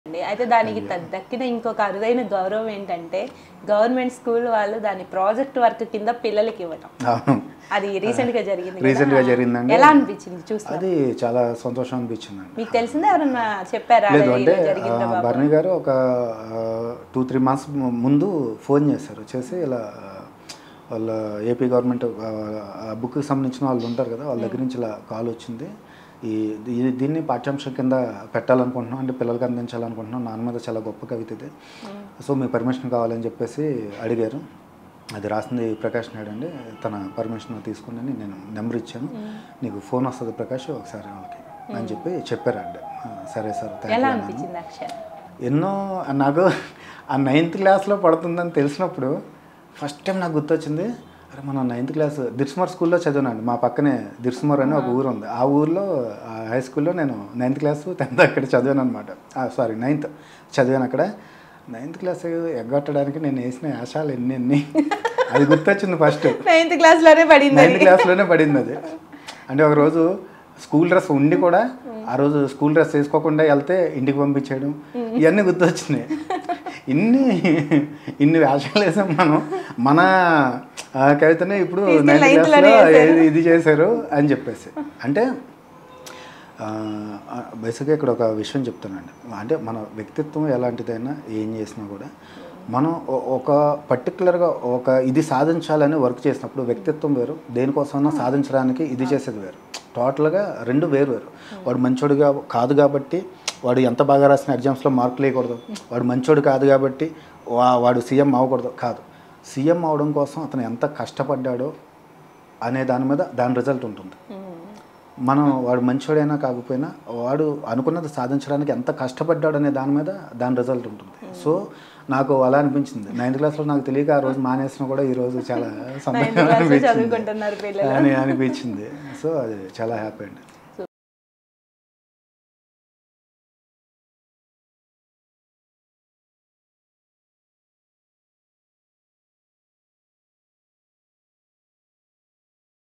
My Novels Have Been Published In AP Books: Writer Prakash Naidu | Vihari Tv
Prakash Naidu Panasakarla, a celebrated writer, actor, and director shares his 9th standard memories and reveals that his novels have been selected by the AP Government for projects. To gain a deeper insight into his remarkable journey, don't miss out on watching the full video.